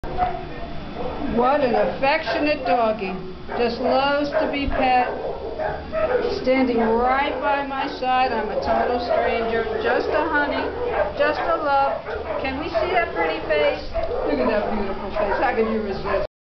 Meet Lucy a Siberian Husky currently available for adoption at Petango.com! 7/3/2015 5:03:42 PM
Let the 4th of July sparkle for you and a Shelter animal. July 1 - 6, 2015 all dogs and puppies can come home with you for $17.76 (normally Adoption fee is $120) with all the benefits of HSTC Adoption included .